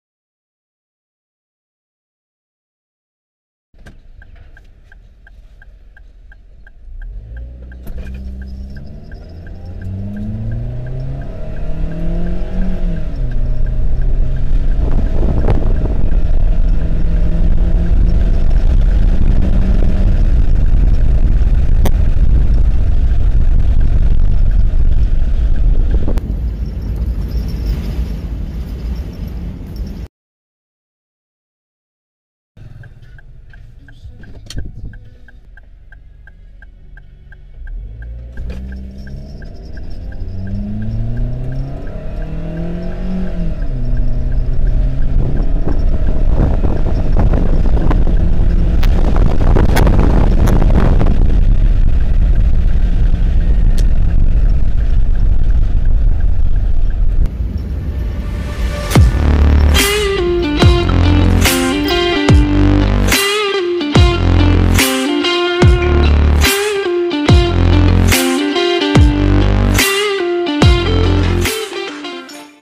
Perodua Bezza 1300cc 0-100km/h Test
This Perodua Bezza Spec :
- Advance spec
- Inline-4 1NR-VE Dual VVT-i engine
- 4 speed automatic transmission
- 94hp (at 6k rpm) 121 nm (at 4k rpm)
- ABS/EBD & TRC